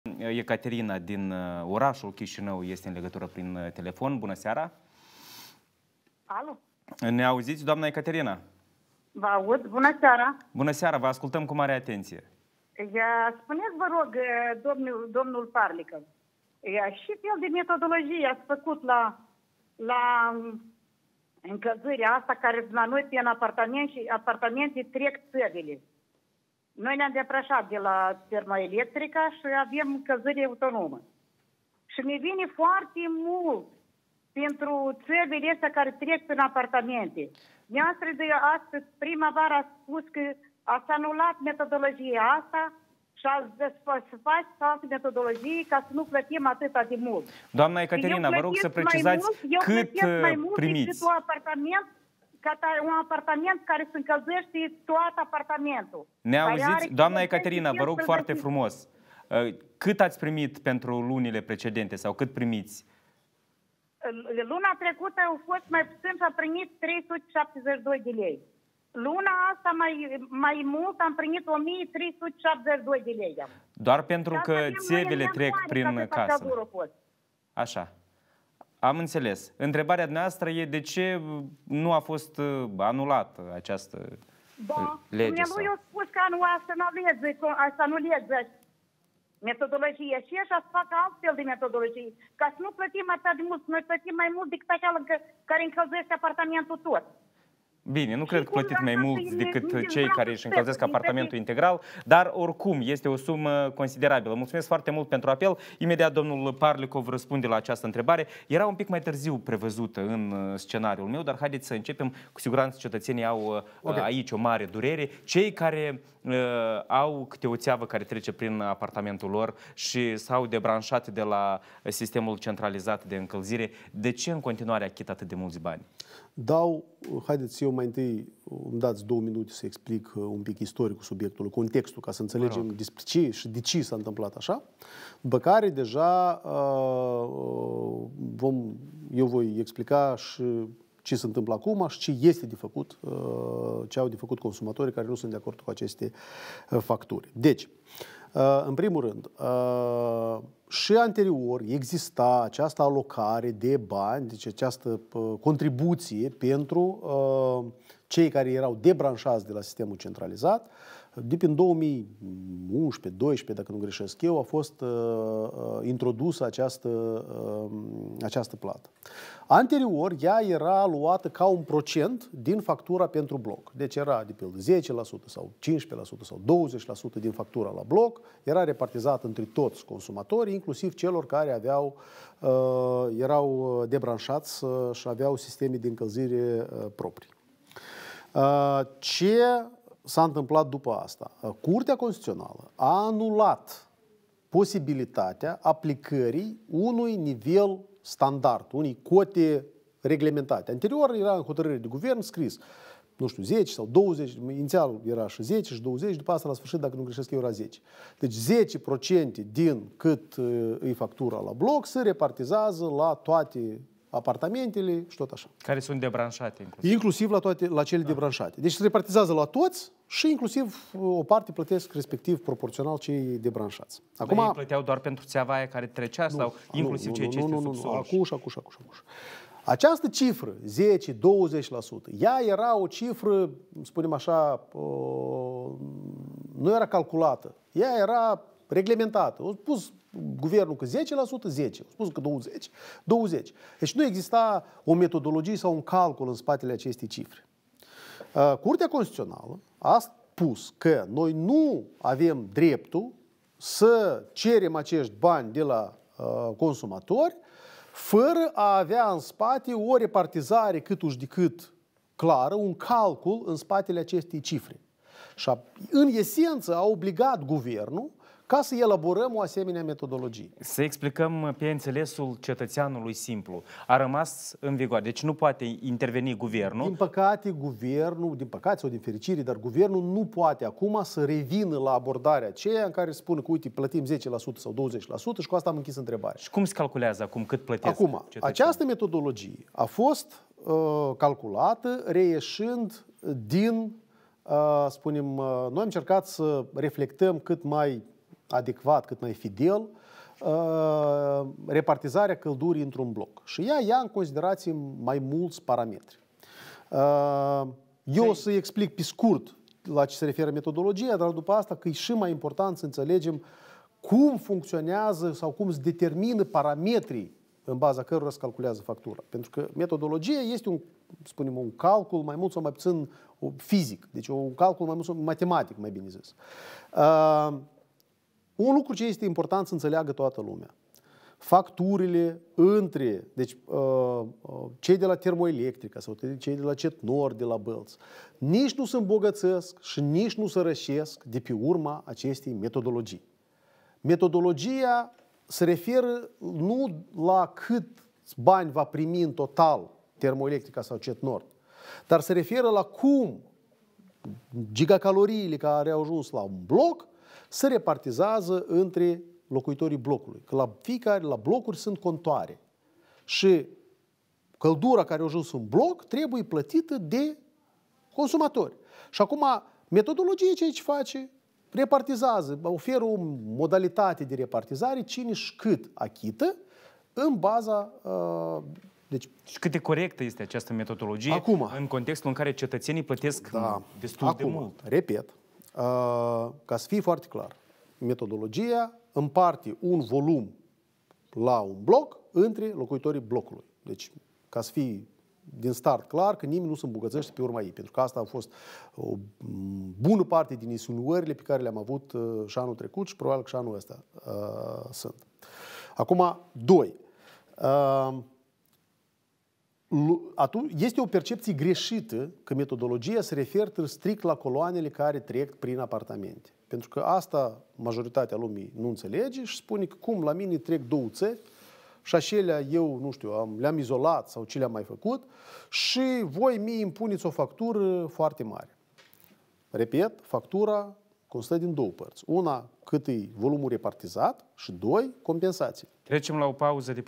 „Luna trecută am plătit 372 de lei, luna asta – 1372 de lei...”
Doamna Ecaterina, din Chișinău nu înțelege de ce trebuie să achite pentru energia termică, din țevile care „tranzitează” apartamentul ei. „Luna trecută am plătit 372 de lei, luna asta – 1372 de lei...”